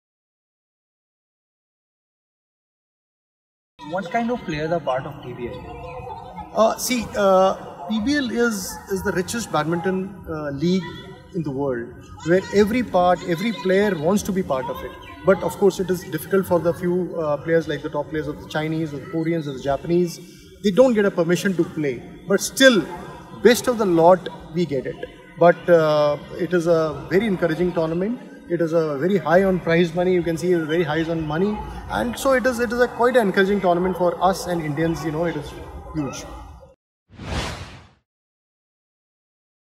In conversation with Ajay Kanwar, head coach, Delhi Dashers! Video 3
What brings the best badminton players in the world to the PBL? Watch Ajay Kanwar, Head Coach, Delhi Dashers, as he sheds light on the toughest badminton league in the world.